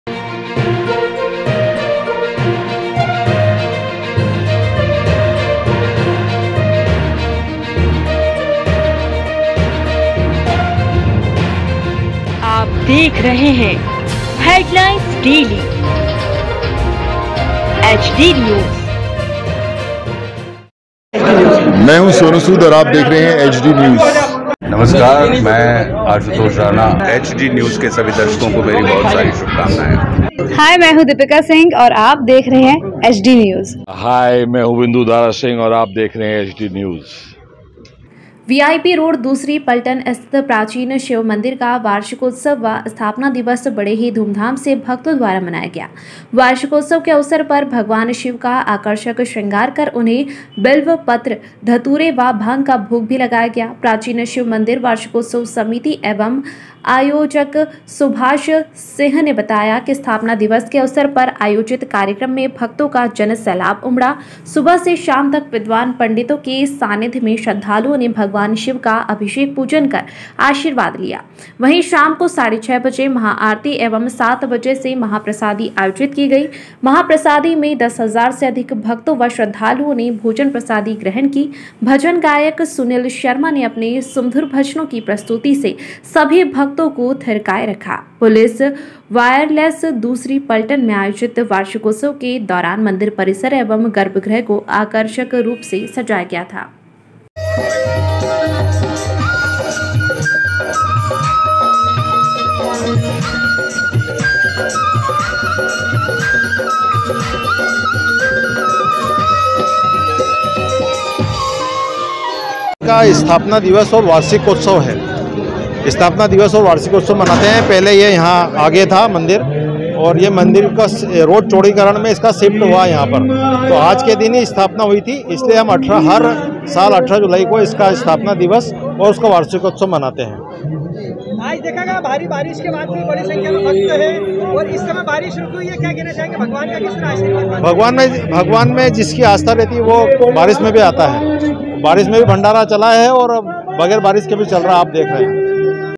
शिव का हुआ आकर्षक श्रृंगार, 8 हजार लोगों ने ग्रहण की महाप्रसादी...वार्षिकोत्सव व
स्थापना दिवस समारोह में उमड़ा भक्तों का सैलाब, मध्यरात्री तक जारी रही भजन संध्या